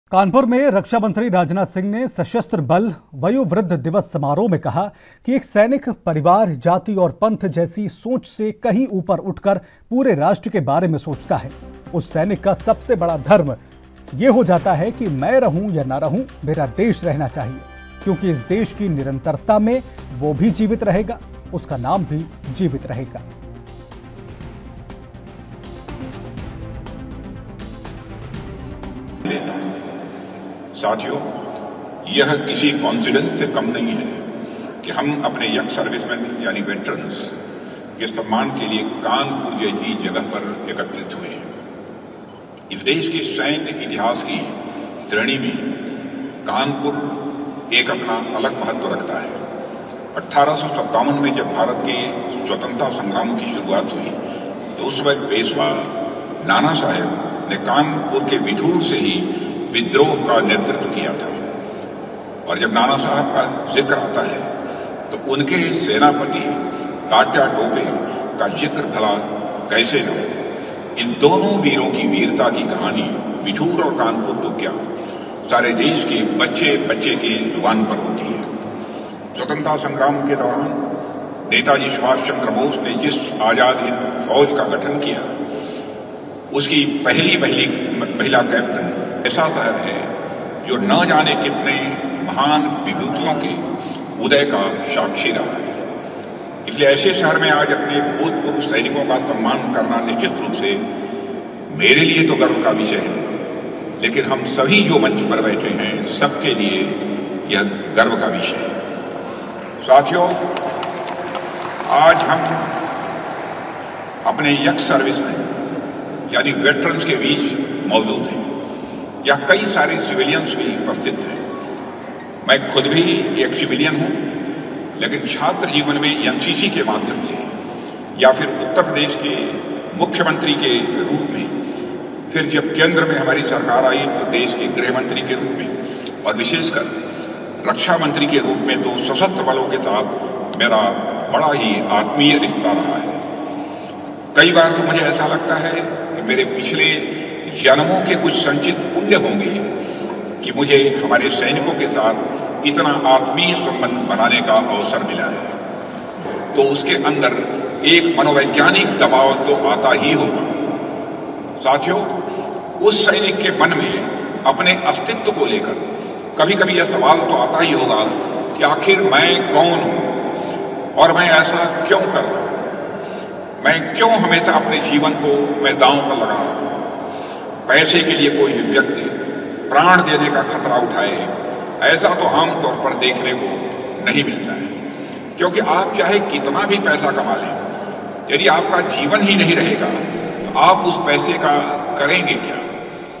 कानपुर, यूपी : सशस्त्र बल वयोवृद्ध दिवस समारोह का आयोजन | ANB NEWS
कानपुर, यूपी : सशस्त्र बल वयोवृद्ध दिवस समारोह का आयोजन
कार्यक्रम में शामिल हुए रक्षा मंत्री राजनाथ सिंह
मैं रहूं या ना रहूं, मेरा देश रहना चाहिए- राजनाथ